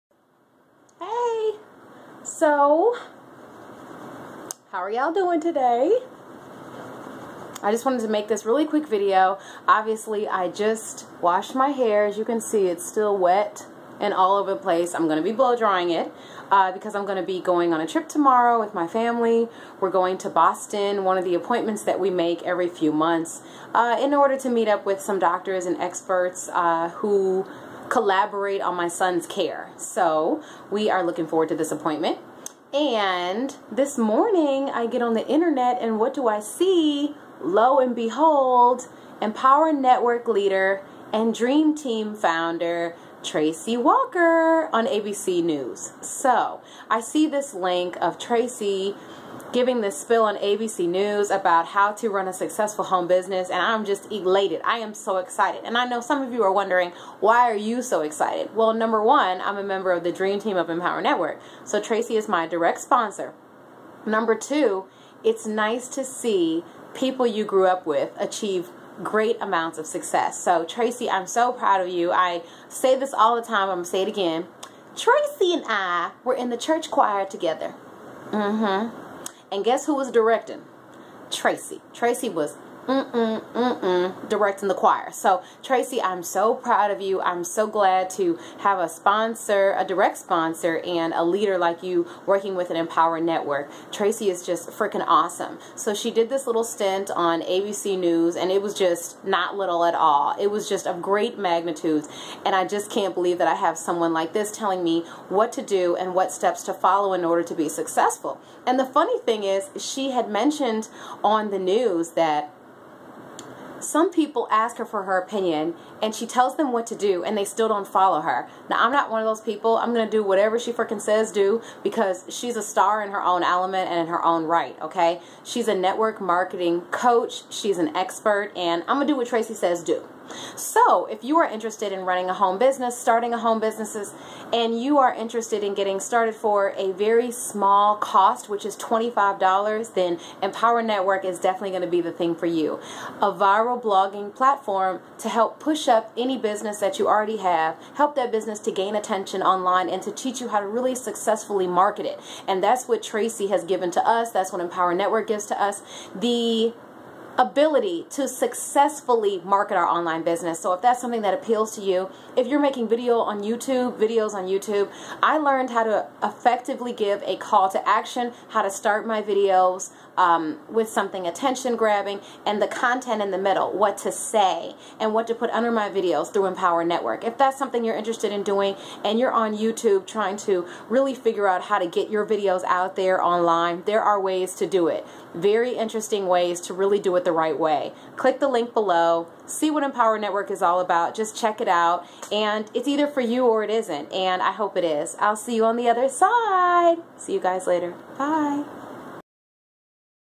How to have a Successful ONLINE Business: Tracey Walker on ABC NEWS!!!
Tracey Walker has taught me how to start my online business and how to market myself online.  I knew NOTHING about how to market through youtube until I followed her lead.

If you are looking to market or expand your business online then click this link! GET STARTED!

Its only $25 to learn Tracey's marketing strategies!
(sorry about the lighting.  It went crazy!